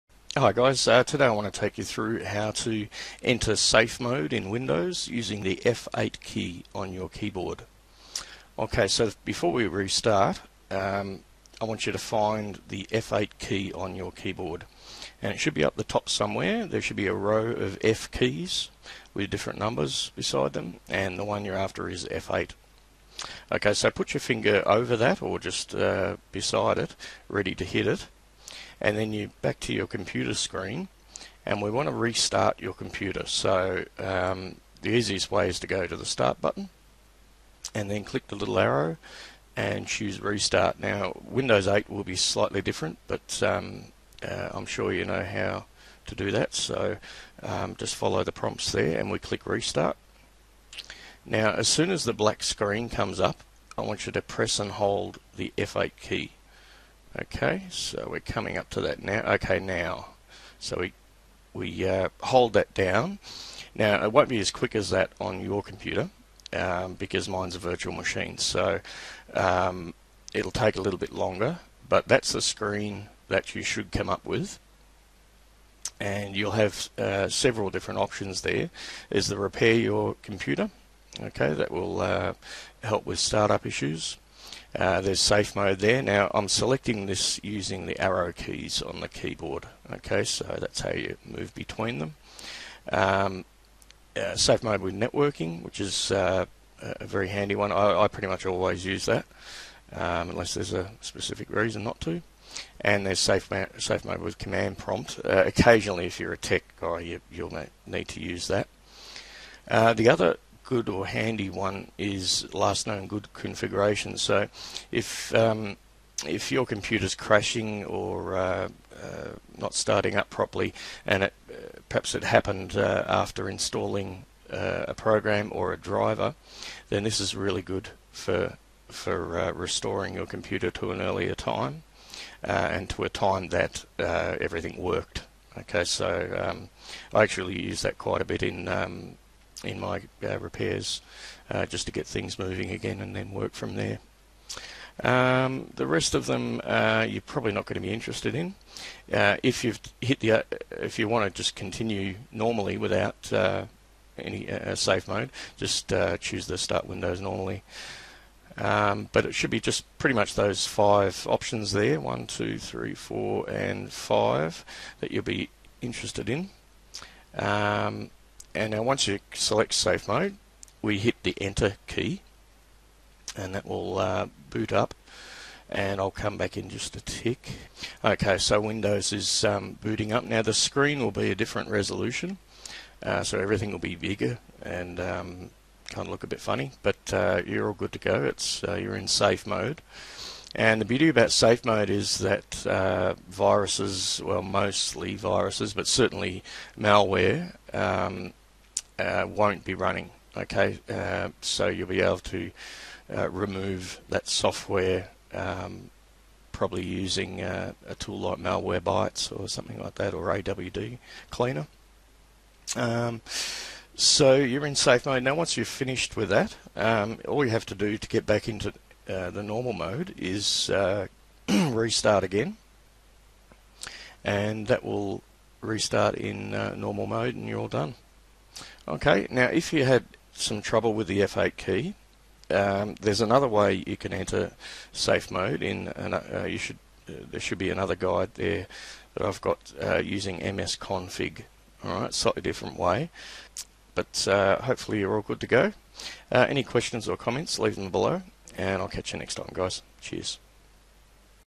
Enter safe mode using f8 key Windows
Sometimes you need to run Windows in Safe Mode which means only the basic Windows services will start leaving everything else stopped. It's handy for troubleshooting a program, removing viruses and malware, and fixing Windows errors.

I will show you how to enter Safe Mode using the F8 key on your keyboard in this video guide.